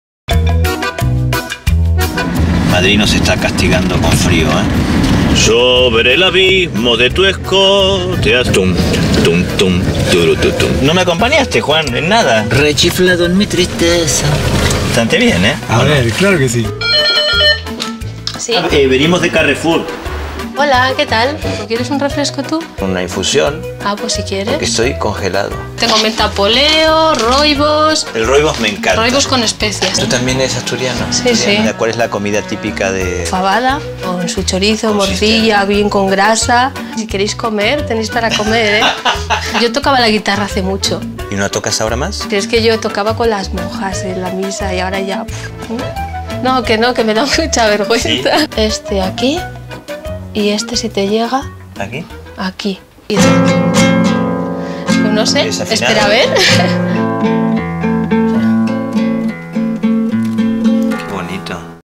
Séptima entrega de Leonardo Sbaraglia en Madrid
En esta séptima entrega, Leonardo descubrirá los secretos de la gastronomía asturiana de mano de Conchi, otra de las diez personas ganadoras del premio de